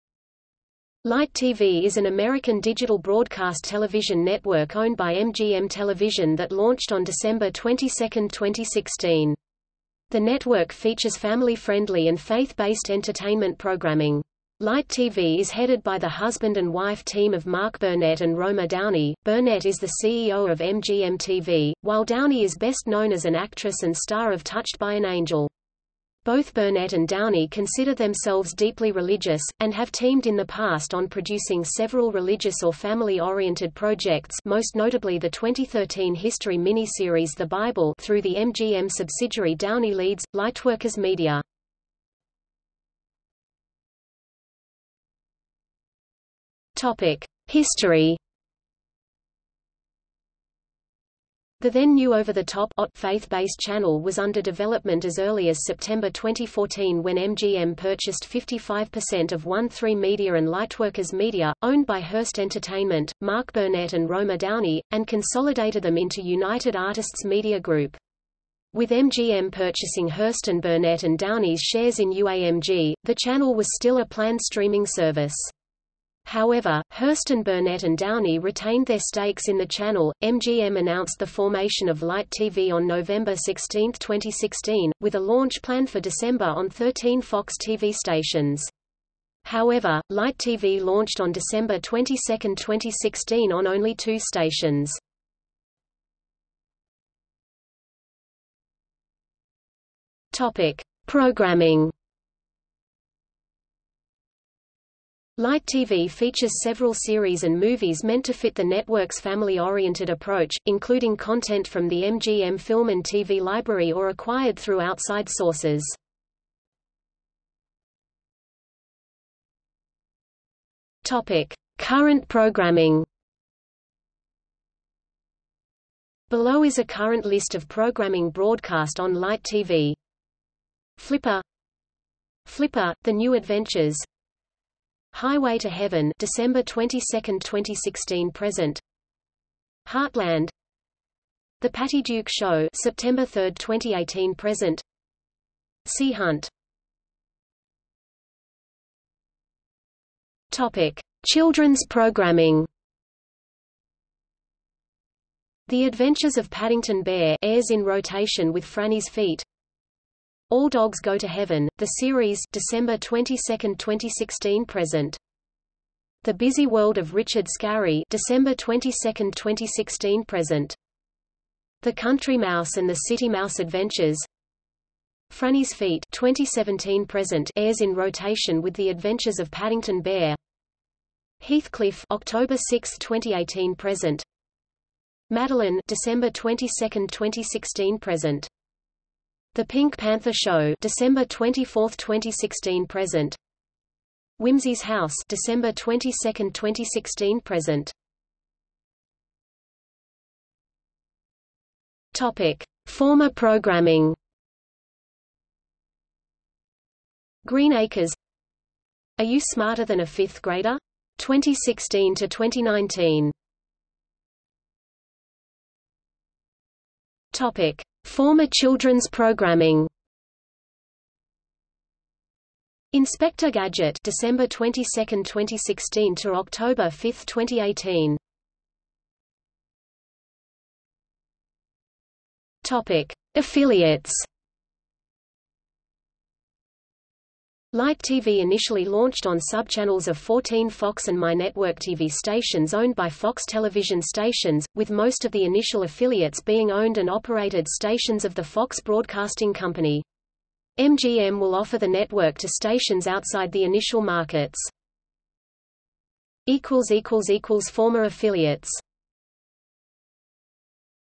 Light TV | Wikipedia audio article
00:00:43 1 History
00:01:36 2 Programming
00:01:57 2.1 Current programming
00:02:27 2.1.1 Children's programming
00:03:18 2.2 Former programming
00:03:34 2.2.1 Former children's programming
00:03:49 3 Affiliates

- Socrates

SUMMARY
Light TV is an American digital broadcast television network owned by MGM Television that launched on December 22, 2016.  The network features family-friendly and faith-based entertainment programming. Light TV is headed by the husband-and-wife team of Mark Burnett and Roma Downey; Burnett is the CEO of MGM TV, while Downey is best known as an actress and star of Touched by an Angel. Both Burnett and Downey consider themselves deeply religious, and have teamed in the past on producing several religious- or family-oriented projects (most notably the 2013 History miniseries The Bible) through the MGM subsidiary Downey leads, Lightworkers Media.